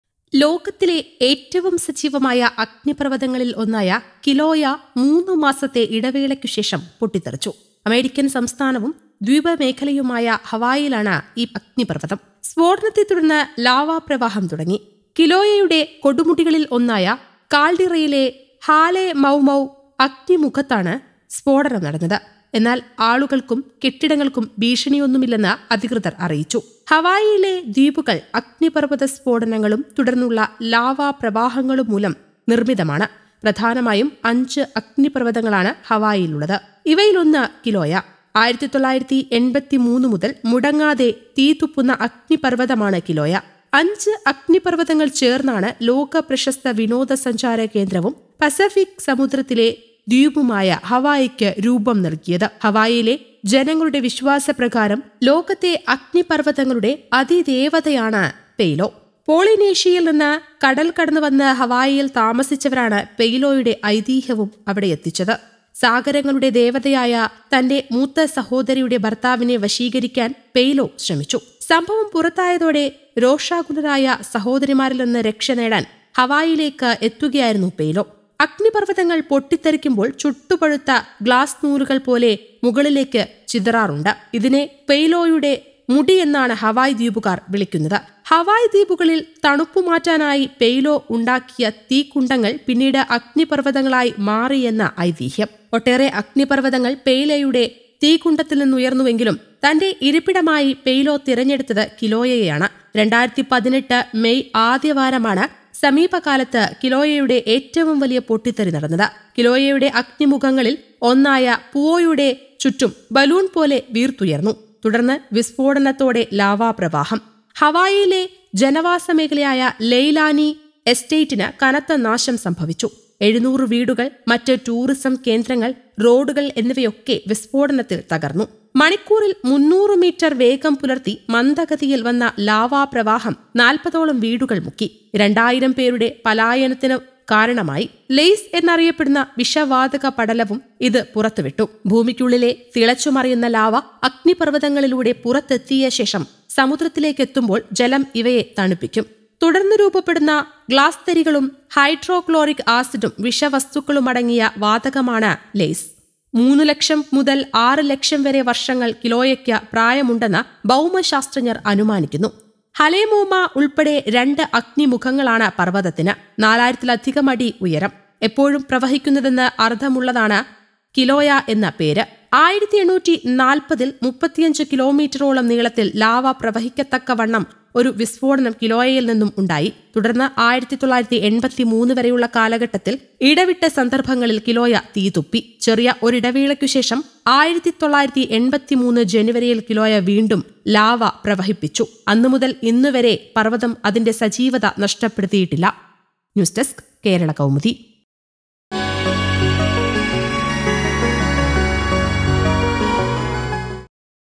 U S Kilauea volcano | തീതുപ്പി കിലോയ അഗ്നിപർവ്വതം, ഹവായ നിറയെ ലാവ
ലോകത്തിലെ ഏറ്റവും സജീവമായ അഗ്നിപർവതങ്ങളിലൊന്നായ  കിലോയ 3 മാസത്തെ ഇടവേളയ്ക്കു ശേഷം പൊട്ടിത്തെറിച്ചു. അമേരിക്കൻ സംസ്ഥാനവും ദ്വീപമേഖലയുമായ ഹവായിയിലാണ്  ഈ അഗ്നിപർവതം. സ്‌ഫോടനത്തെത്തുടർന്ന് ലാവാപ്രവാഹം തുടങ്ങി. കിലോയയുടെ കൊടുമുടികളിലൊന്നായ കാൽഡിറയിലെ ഹാലെമൗമൗ  അഗ്നിമുഖത്താണു സ്‌ഫോടനം നടന്നത്. എന്നാൽ ആളുകൾക്കും കെട്ടിടങ്ങൾക്കും ഭീഷണിയൊന്നുമില്ലെന്ന് അധികൃതർ അറിയിച്ചു. ഹവായിയിലെ ദ്വീപുകൾ അഗ്നിപർവത സ്‌ഫോടനങ്ങളും തുടർന്നുള്ള ലാവാപ്രവാഹങ്ങളും മൂലം നിർമിതമാണ്. പ്രധാനമായും 5 അഗ്നിപർവതങ്ങളാണ് ഹവായിയിലുള്ളത്. ഇവയിലൊന്നാണ് കിലോയ.